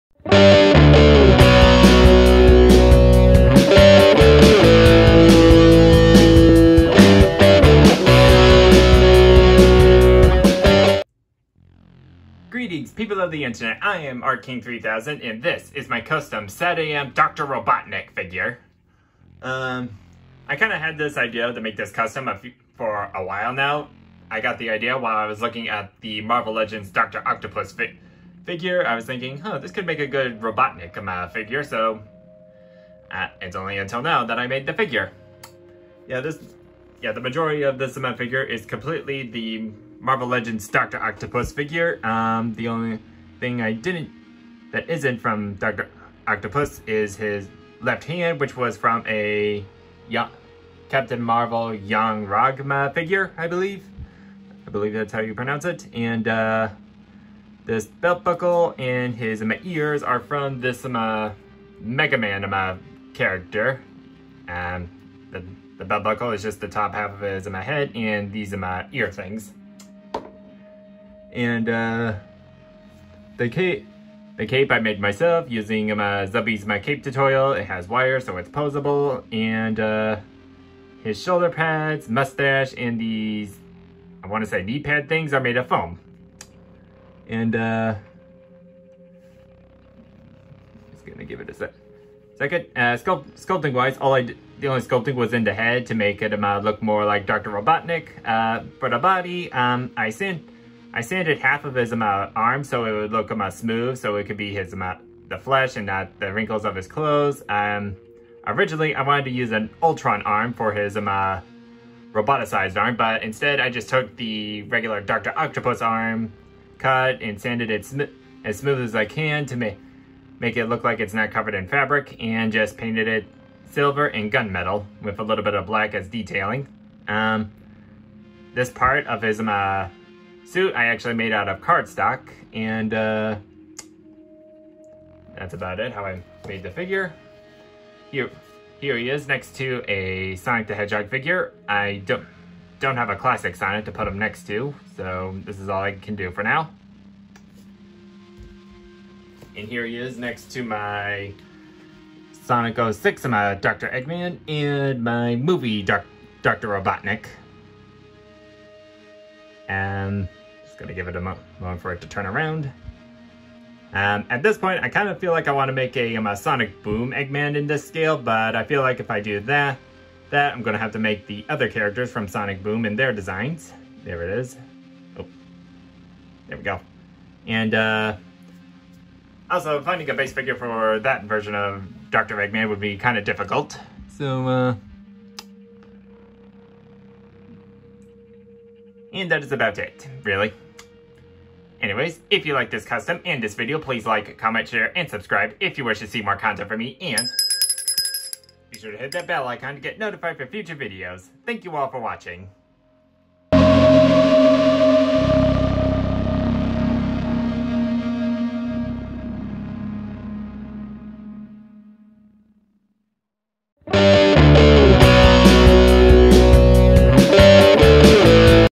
Custom Satam Dr. Robotnik Custom
My custom of Dr. Robotnik from the Sonic the Hedgehog Saturday morning cartoon.